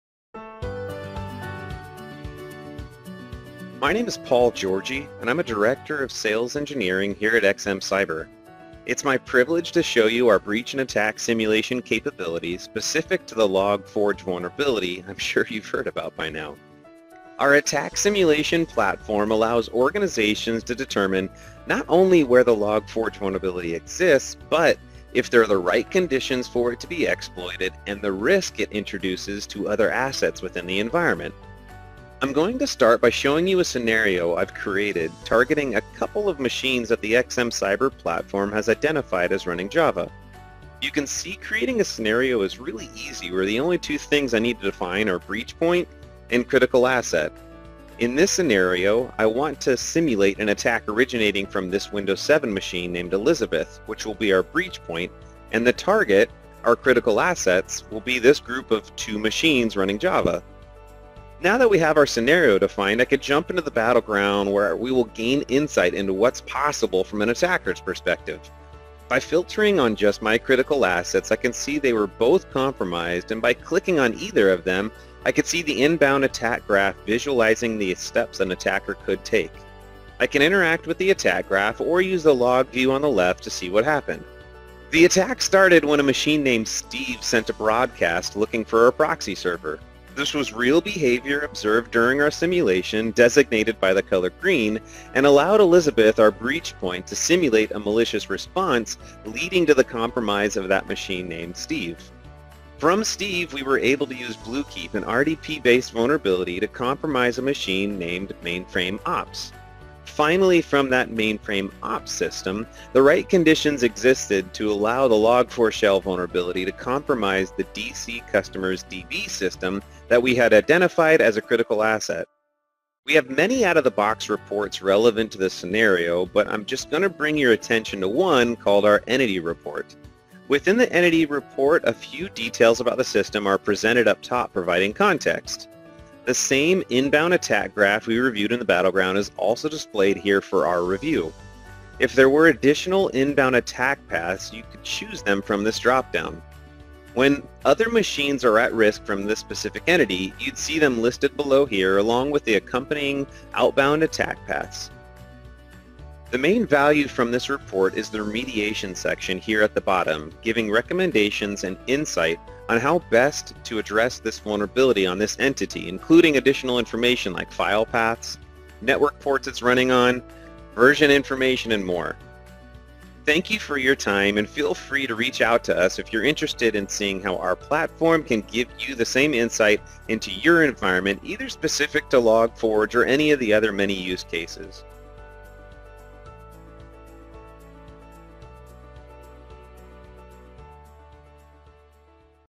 Go beyond Log4Shell and see the entire attack path with XM Cyber
We know you're working tirelessly to get ahead of the log4j vulnerability. Here at XM Cyber, we can help you prioritize your remediation efforts to not only identify Log4j across your environment but help you understand where it can be exploited in an attack path so you can fix those exposures first. Watch this video to learn how